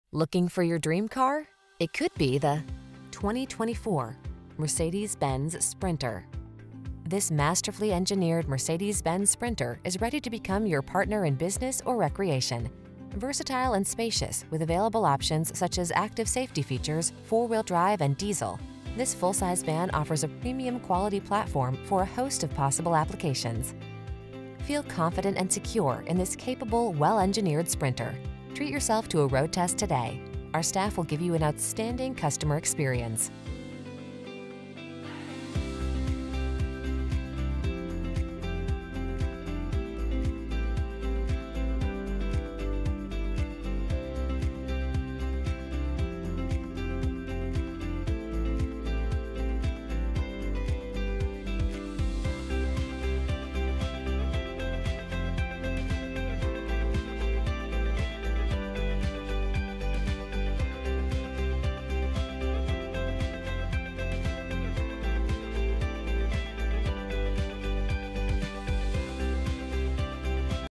2024 Mercedes-Benz Sprinter_3500 Cab Chassis 144 WB Duluth, Lawrenceville, Cumming, Johns Creek...
New 2024 Mercedes-Benz Sprinter_3500 Cab Chassis 144 WB available at Mercedes-Benz of Atlanta Northeast located in 1705 Boggs Rd, Duluth, GA, 30096.
Stock#: S1823 - VIN#: W1X8N23Y2RN290937

Servicing the Duluth, Lawrenceville, Cumming, Johns Creek, Atlanta area.